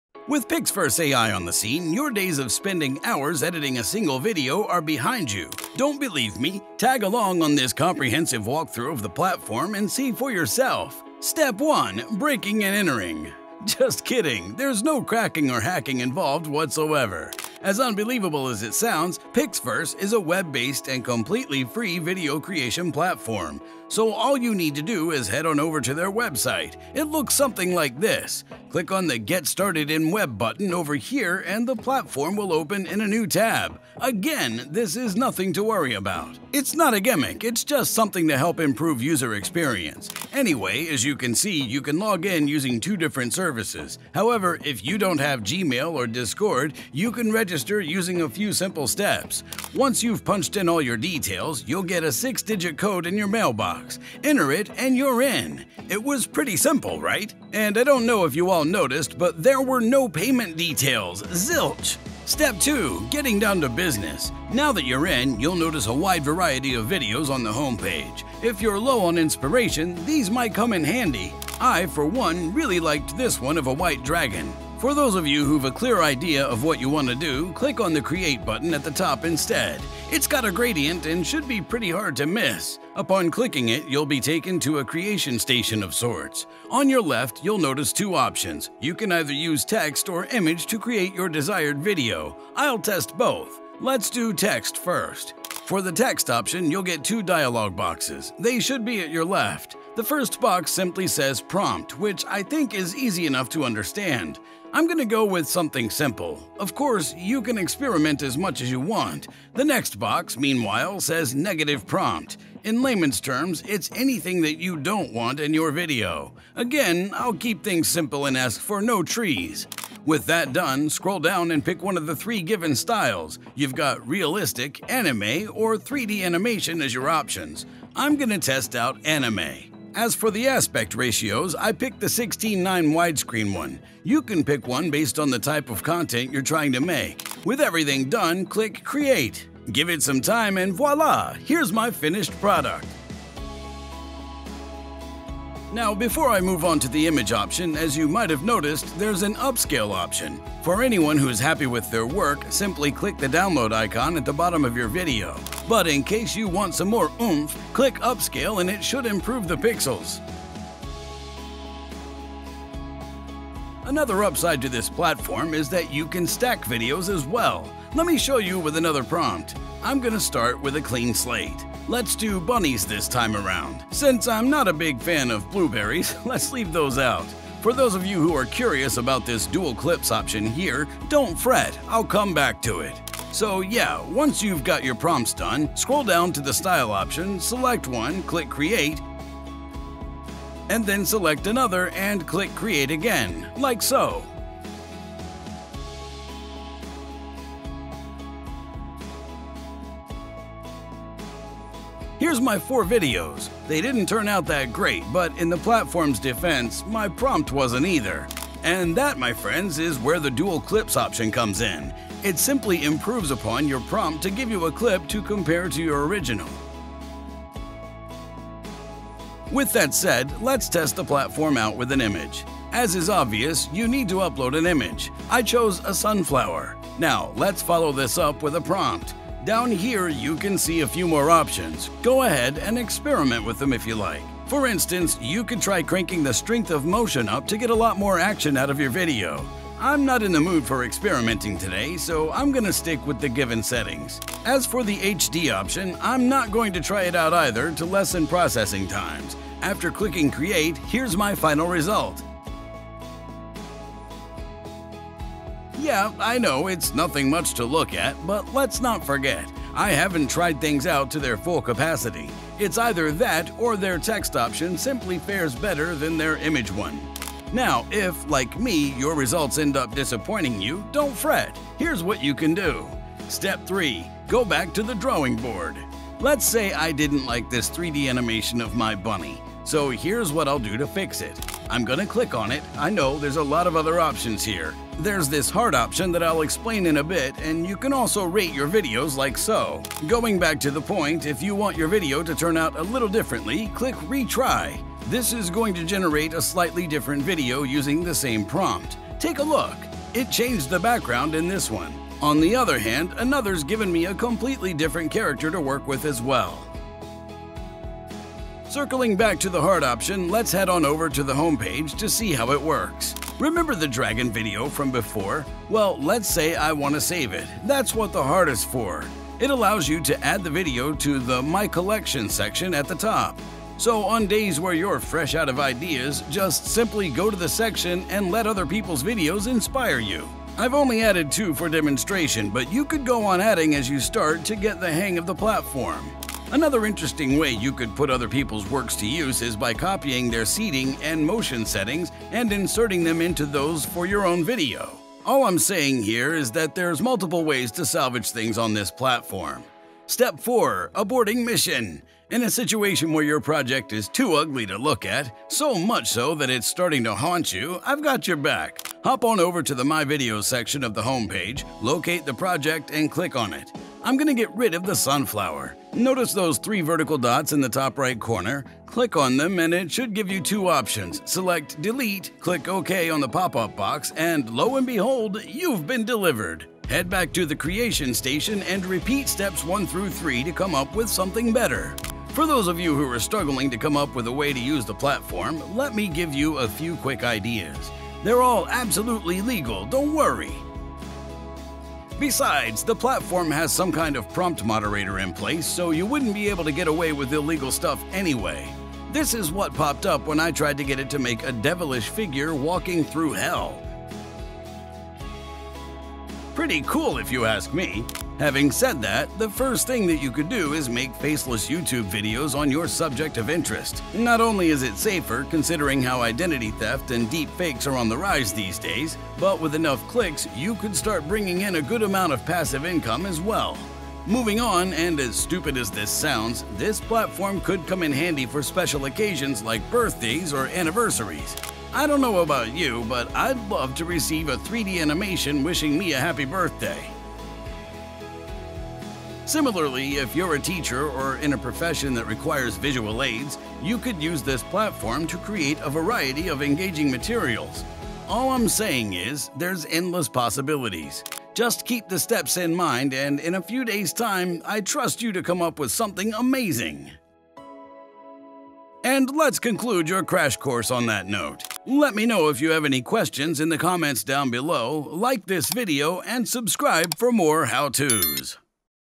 Pixverse Ai Tutorial Free Ai Video Generator
Pixverse AI Tutorial Free AI Video Generator is a comprehensive video guide designed for creators eager to delve into the vast capabilities of Pixverse AI, a cutting-edge free AI video generator. Dive into the world of Pixverse AI by learning how to harness its power, from creating images through Pixverse AI Discord to utilizing Pixverse Magic Studio for innovative projects. Follow a detailed step-by-step process to convert text to images and videos effortlessly, and even craft AI meme faces with ease. Whether you're enhancing your videos at no cost or venturing into the realm of generative AI, this Pixverse tutorial is your ultimate resource. Explore the flexibility of Pixverse AI, unlock new creative horizons with the Pixverse video generator, and perfect the art of transforming text to images and videos using a variety of AI tools.